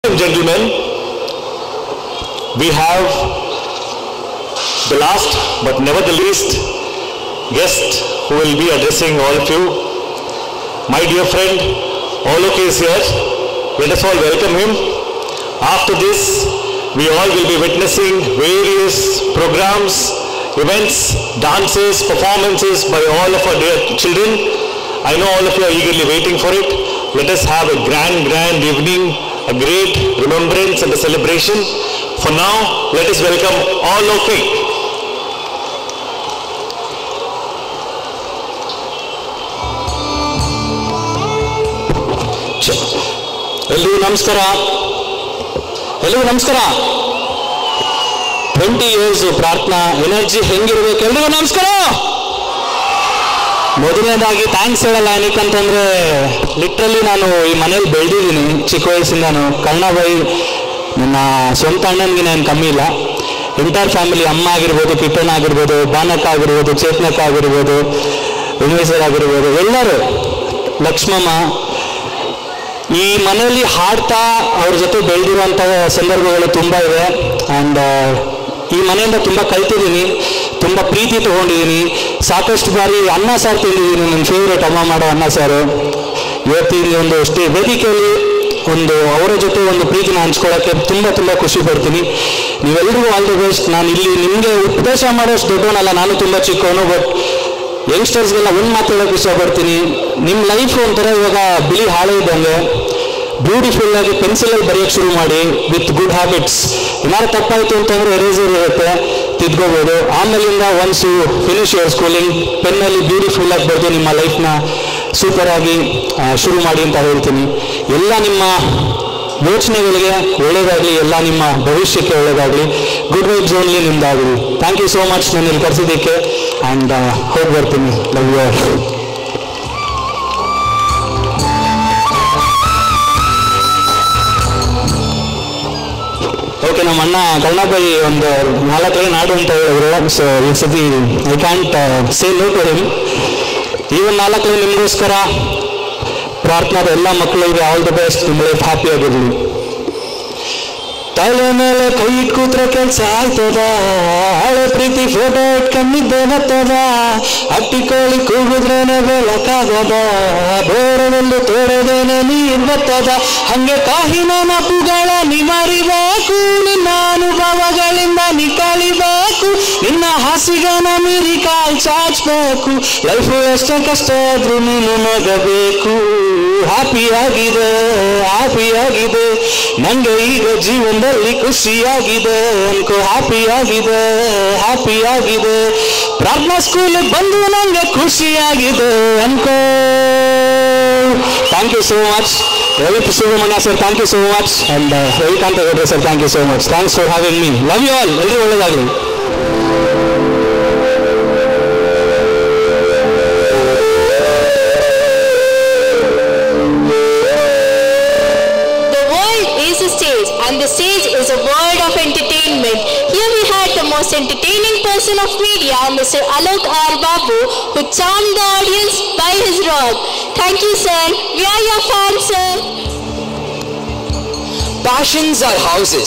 Prarthanaschool | RAVIUTSAVA | Singer Alok R Babu Speech
Popular rapper & singer Alok R Babu takes the stage on Raviutsava to give a vote of thanks. The rapper was greeted with cheers and excitement from the students and audience, he thanked the school for the invitation and expressed his admiration for the hard work and dedication of the students and teachers. The rapper shared his personal anecdotes, experiences, wisdom from their journey with Ravi Belagere. The rapper also touched upon topics such as the importance of education, pursuing one's passion and overcoming obstacles.